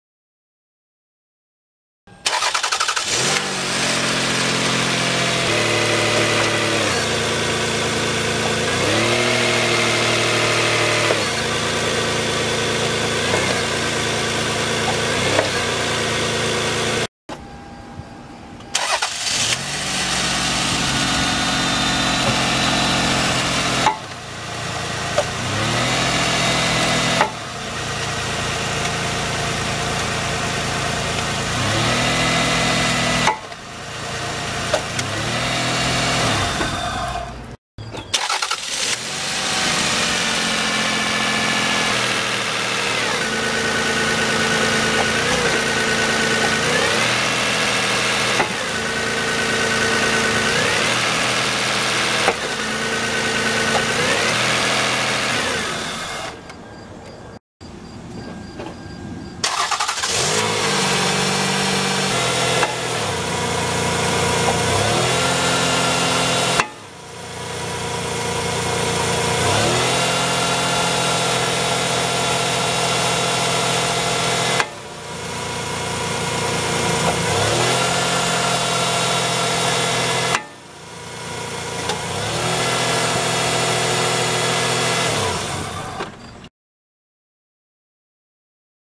Broken Rear Engine Mount Camry V6
Rear engine mount failure on the 1999 V6 Solara.
The bushing is cracked allowing the engine to move a lot when shifting gears from drive to reverse.

This causes a clunk sound, and a jerk throughout the car.

The other mounts seem fine.